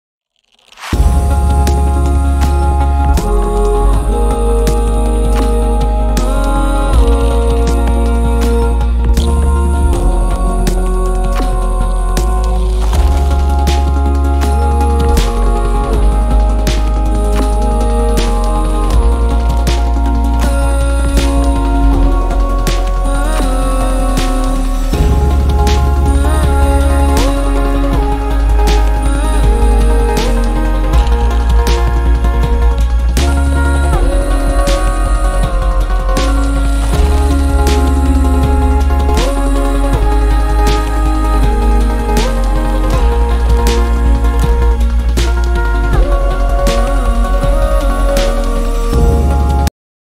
4k Drone Footage ❤️ Mountain Lake ❤️ Aerial View ❤️ Mountain Reflection
Beautiful 4k footage of a mountain lake from above